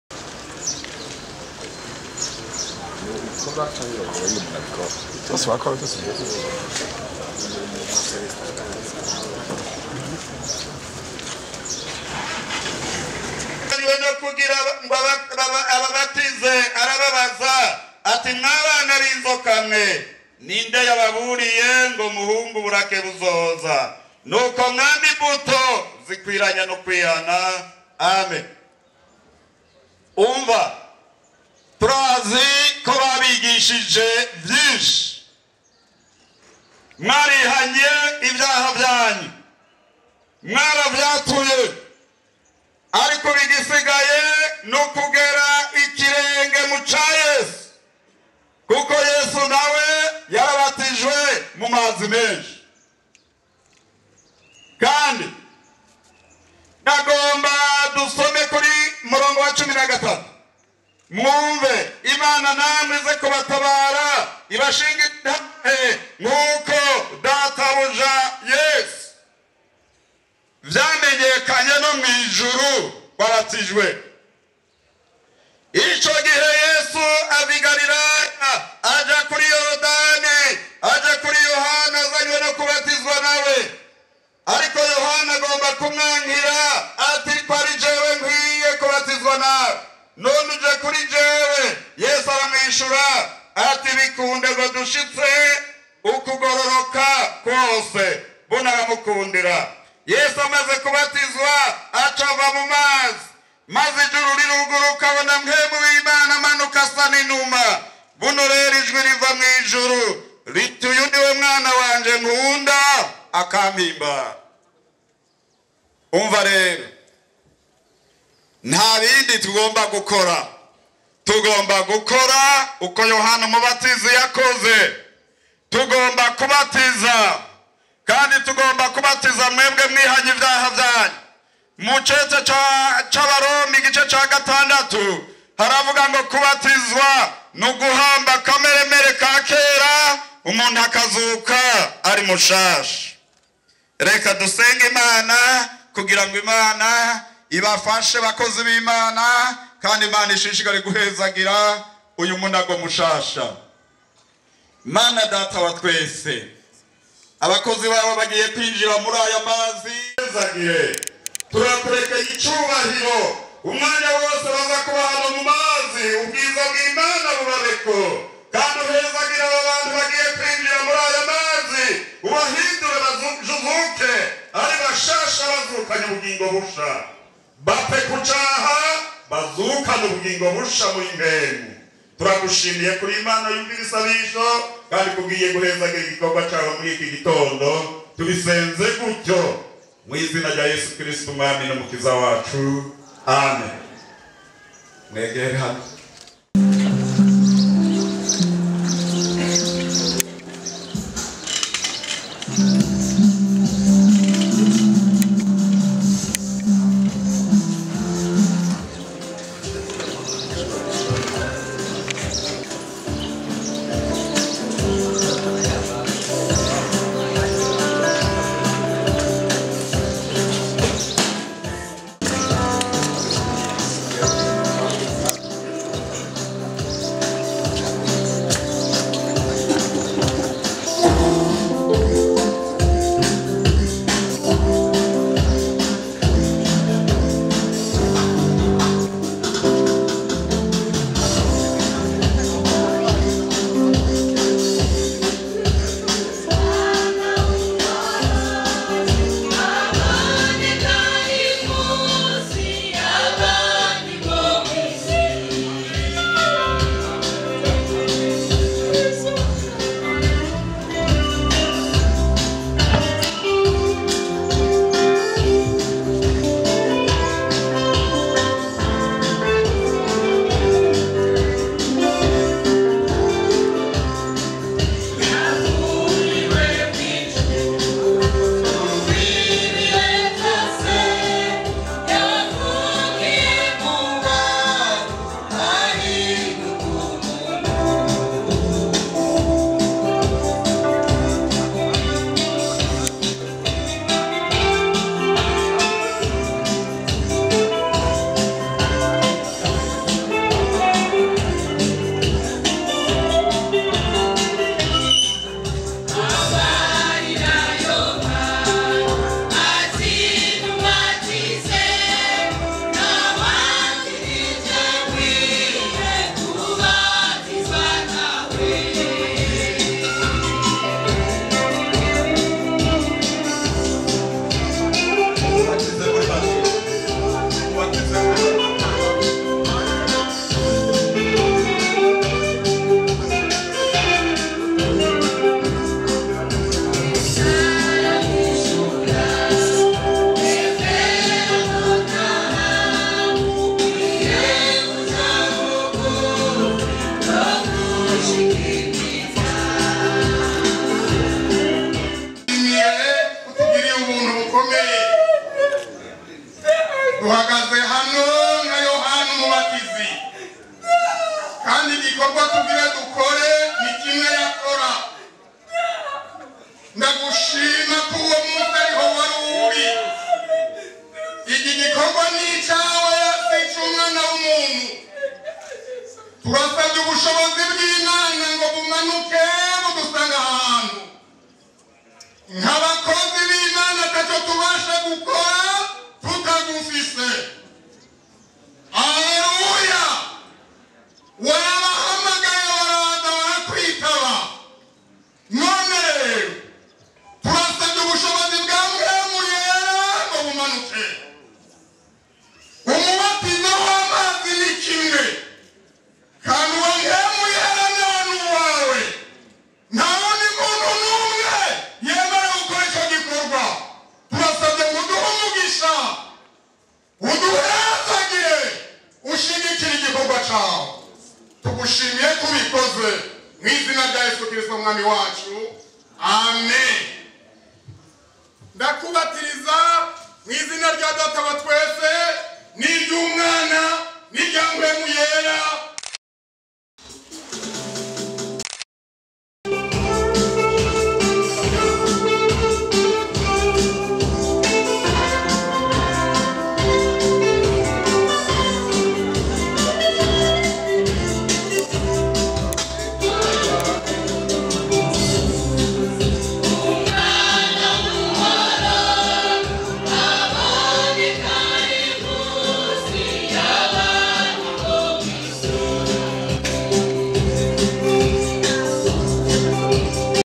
SOCARTIE (Bujumbura - Socartie). IJAMBO RYASOMWE IMBERE YUKO ABAKOZI B'IMANA BATANGURA KUBATIZA
KURIKIRA IJAMBO REV. PASTEUR RUKWIRIRIKANYA YASOMEYE KU RUGOMERO IMBERE YUKO ABAKOZI BINJIRA MU RUGOMERO RWO KUBATIRIZA.